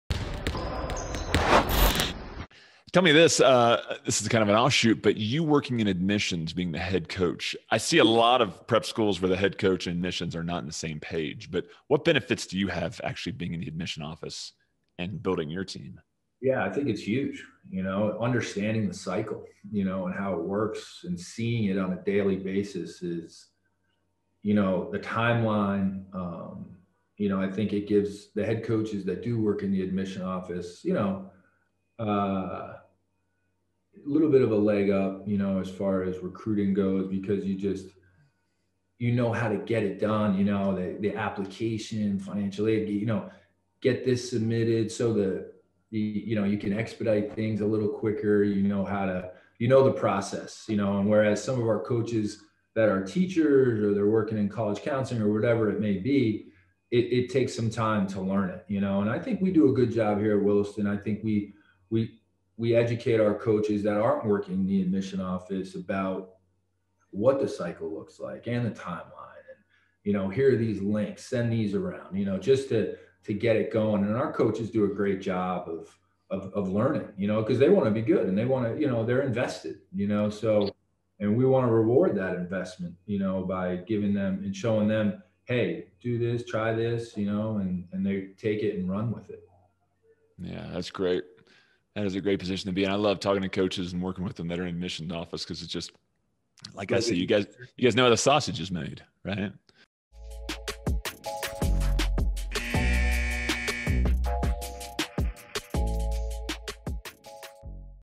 How Does a Prep School Basketball Program Benefit From A Coach Working in the Admission Department?
Whenever you can find a prep school coach who also works in the admission department, a player has an advantage of getting an accurate answer on whether there is a roster spot or financial aid available.  Some athletic departments and coaching staffs are not on the same page and it can be detrimental in getting student athletes admitted with the right amount of financial aid.  Schools where coaches work in the admission departments, take away a lot of the surprise.